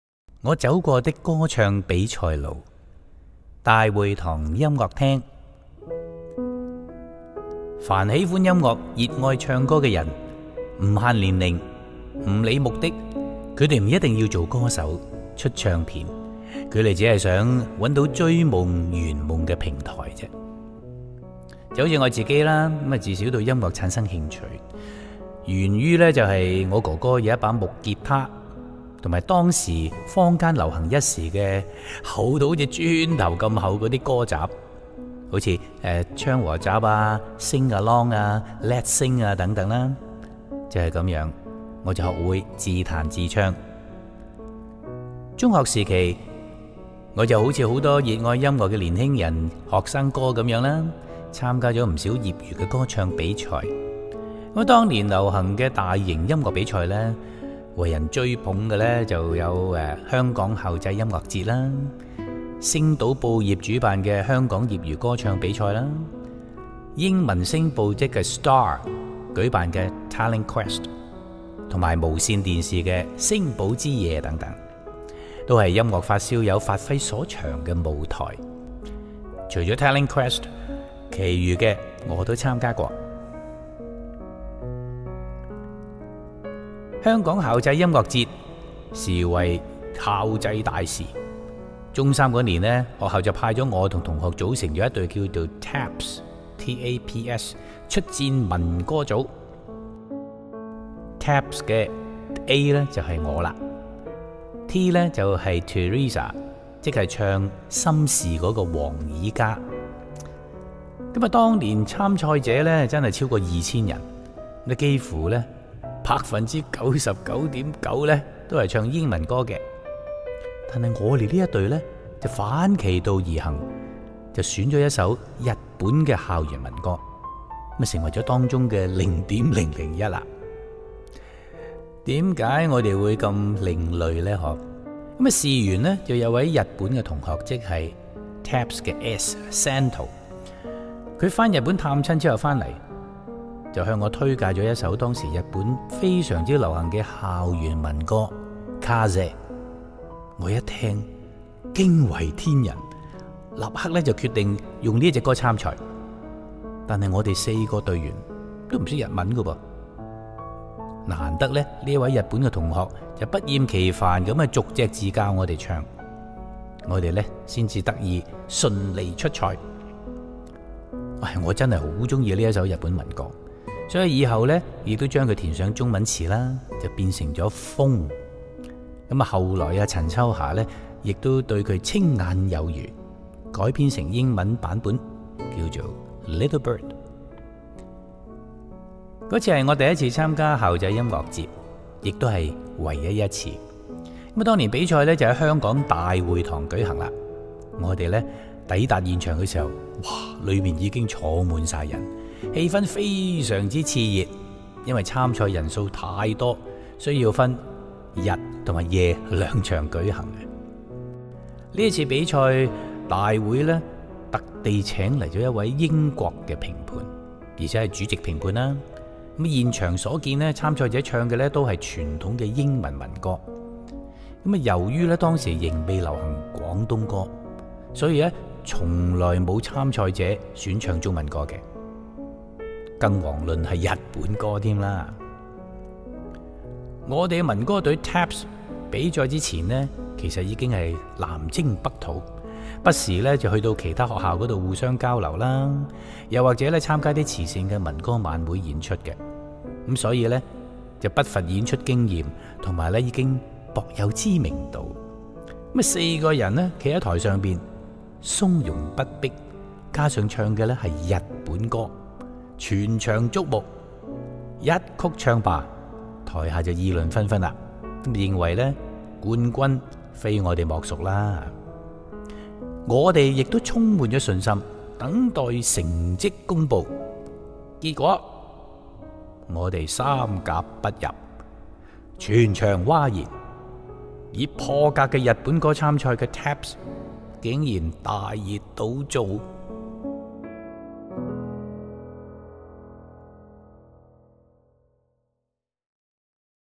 區瑞強：我走過的歌唱比賽路（一）大會堂音樂廳
凡喜歡音樂、熱愛唱歌者，不限年齡、不管目的，他們不一定要當歌手、出唱片，他們只想找到追夢、圓夢的平台。
就好像我自己，自小對音樂產生興趣，源於哥哥那一把木結他，和當時坊間流行一時厚如磚頭的歌集（例如倡和集、Sing Along、Let’s Sing)，就這樣，我學會自彈自唱。
中學時期，我一如許多熱愛音樂的年輕人、學生哥，參加不少業餘歌唱比賽，當年流行的大型音樂比賽，為人追捧的便有香港校際音樂節、星島報業主辦的香港業餘歌唱比賽、英文星報（Star）舉辦的「Talent Quest」，以及無綫電視的《聲寶之夜》等，都是音樂發燒友發揮所長的舞台。除了「Talent Quest」，其餘的我都參加過。